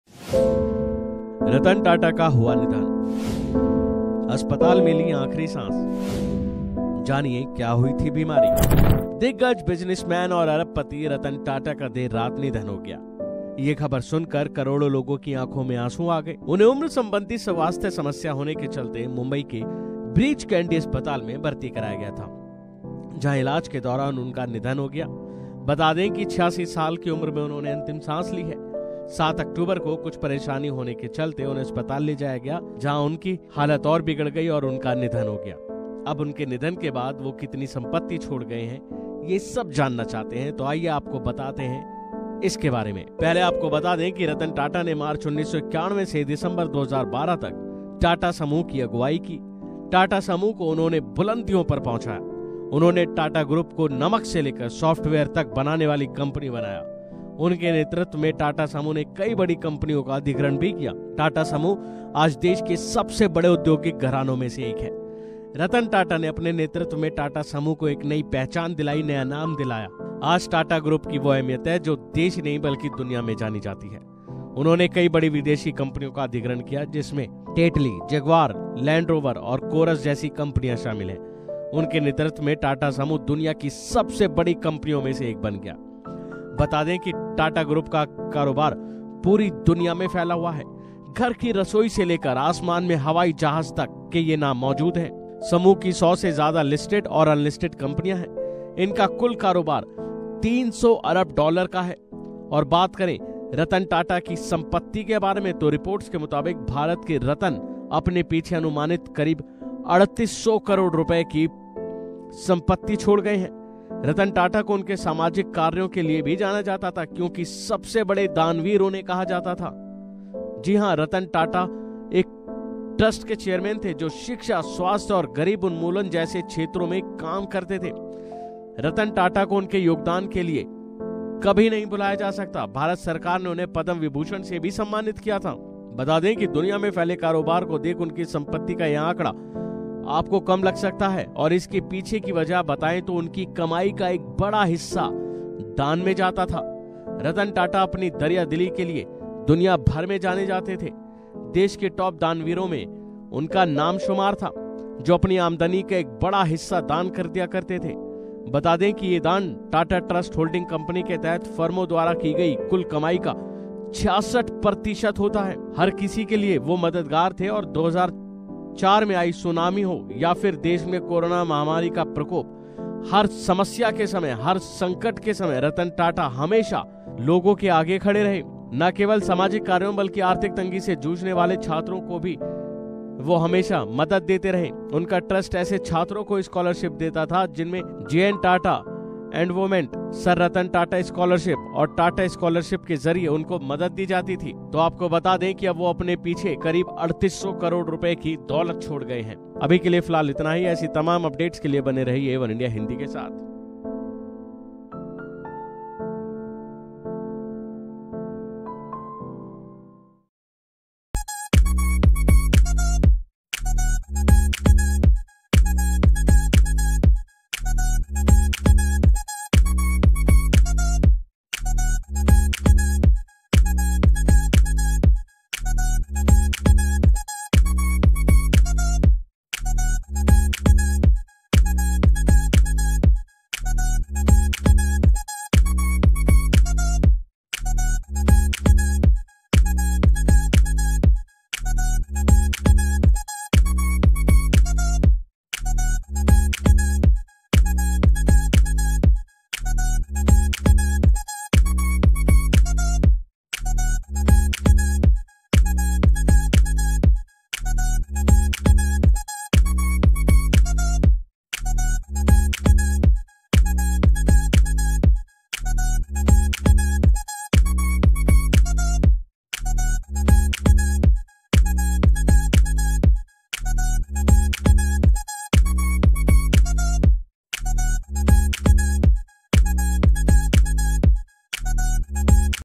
The Legacy of Ratan Tata: Lessons from a Life Well Lived
We Lost One More India's Kohinoor | Ratan Tata Indian Business Tycoon Passed Away

Remembering Ratan Tata: A Legacy of Leadership and Compassion
Description:

In this heartfelt tribute, we honor the life and contributions of Ratan Tata, the visionary industrialist and philanthropist who transformed the Tata Group into a global powerhouse.

Join us as we reflect on his remarkable journey, from his early days to his tenure as chairman, and explore his profound impact on Indian industry, social initiatives, and beyond. We’ll share inspiring stories, key achievements, and the values he championed, including innovation, ethics, and social responsibility.

Let us celebrate the legacy of a man whose kindness and leadership have left an indelible mark on the world. Ratan Tata's vision continues to inspire generations, reminding us all of the power of integrity and compassion in business and life.